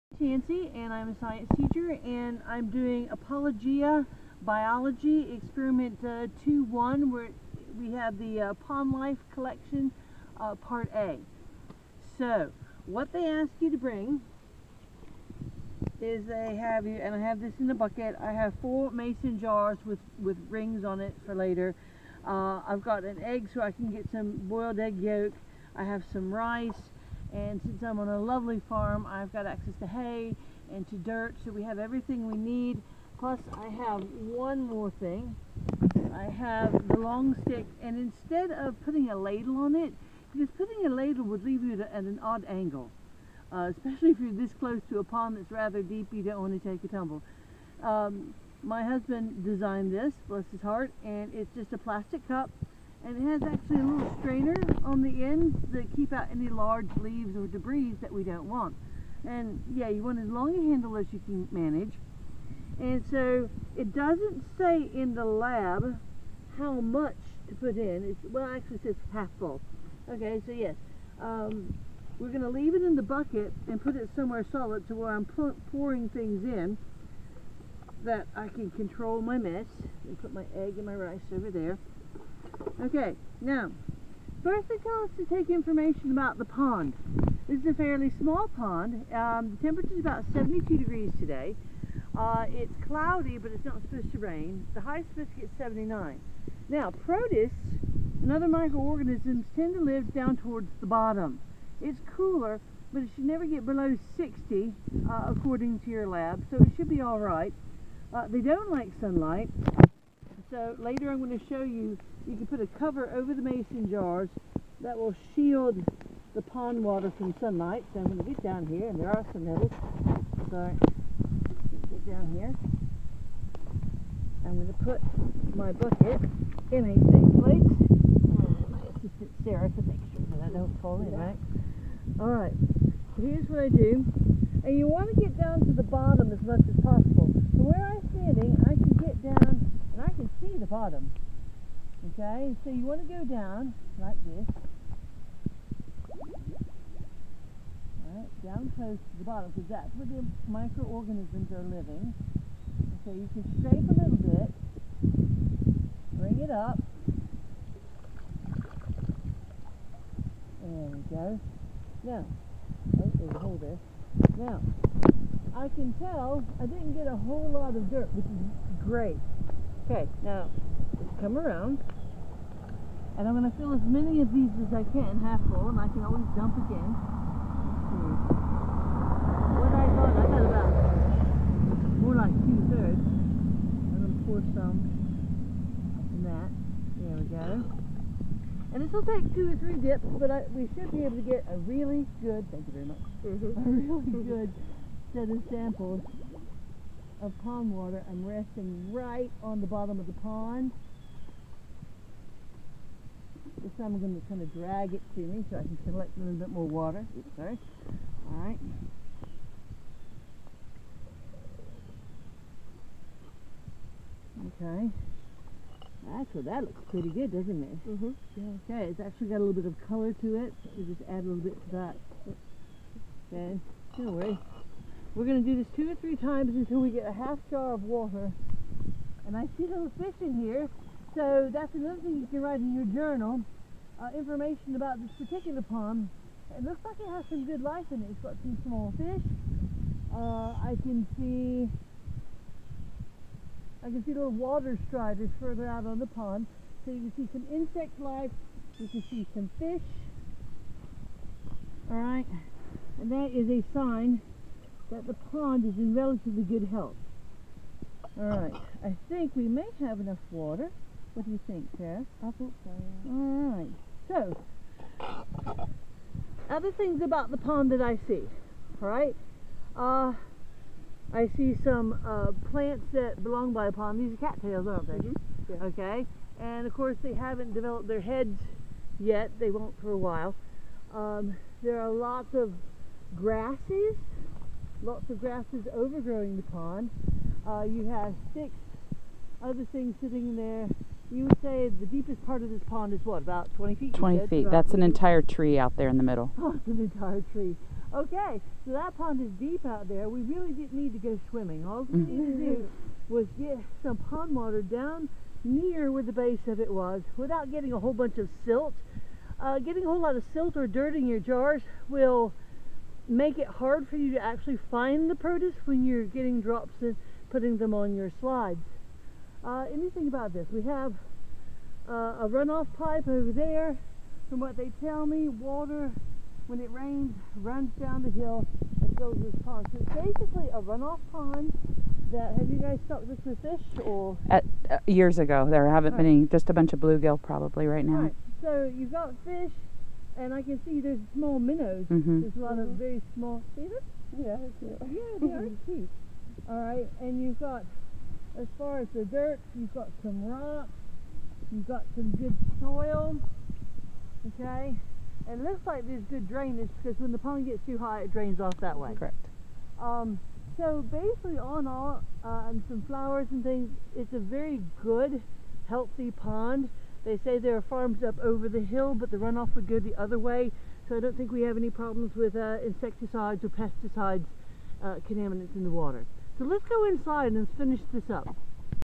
Apologia Biology Lab 2.1, Part 1
This is a video for Apologia Biology Lab 2.1, Entitled Pond Life Part A. I went to a friend's farm to collect specimens from their pond. This part deals with the collection of the samples.